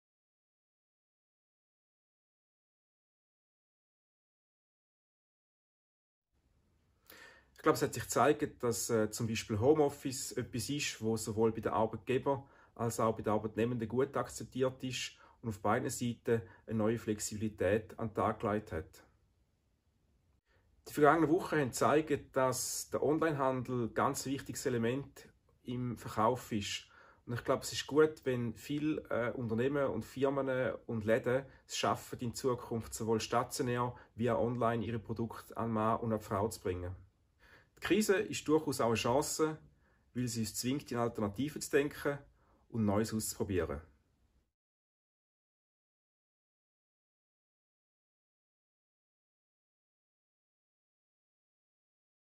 «Never waste a good crisis»
Das sinngemässe Zitat «Verschwende niemals eine gute Krise» wird Winston Churchill zugeschrieben, dem Premierminister, der Grossbritannien durch den Zweiten Weltkrieg geführt hat. Jérôme Müggler, Direktor der @IHK Thurgau, nimmt es als Leitsatz für seine Gedanken zu all den Veränderungen, die das Coronavirus mit sich gebracht hat.